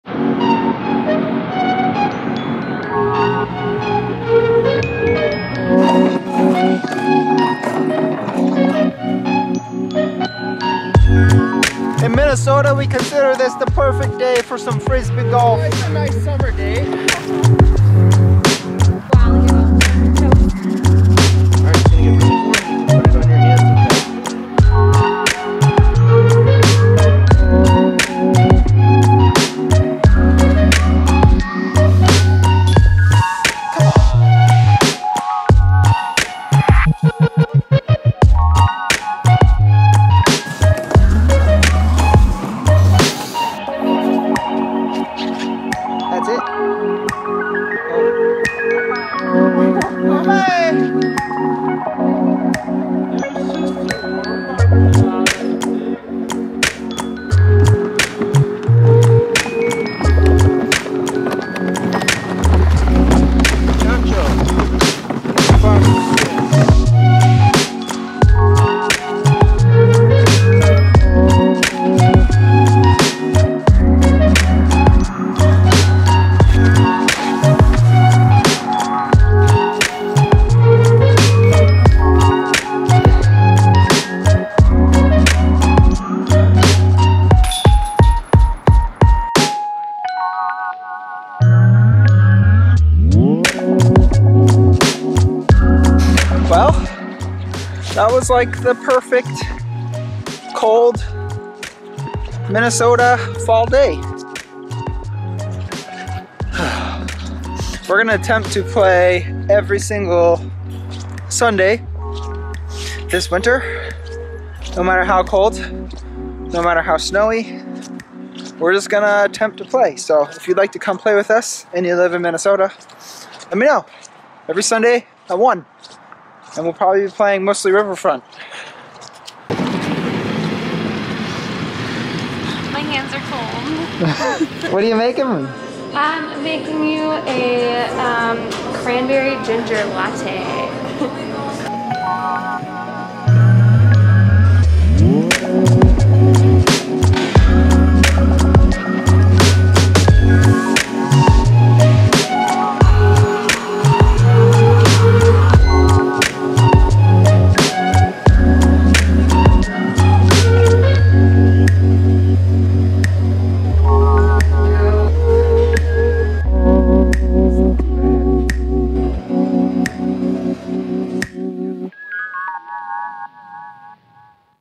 the perfect fall day
When you live in a place that snows for half of the year, you need to make the most of every opportunity to get outside as much as possible.